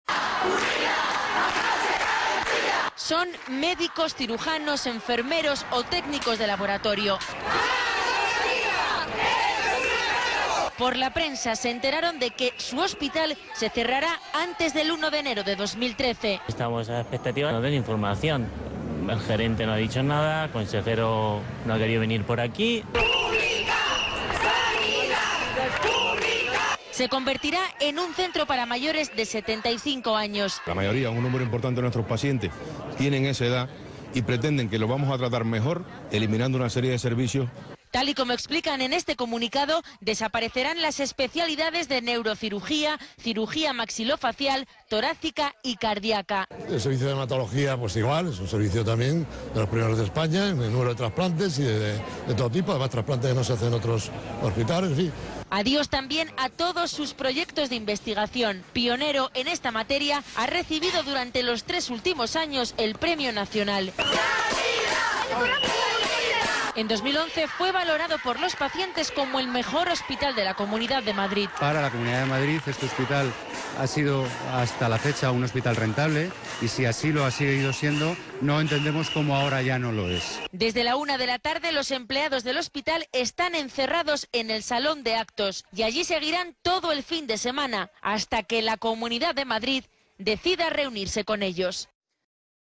El Hospital Princesa cierra las puertas
Los empleados de la Princesa se encierran en el hospital ante el inminente cierre
Los empleados del hospital de la Princesa se han encerrado, por turnos, en el salón de actos y allí seguirán todo el fin de semana. Se han enterado por la prensa que su hospital se cerrará de forma inminente. Por el momento, nadie se ha reunido con ellos y sólo saben que antes del 1 de enero de 2013 el hospital será desmantelado.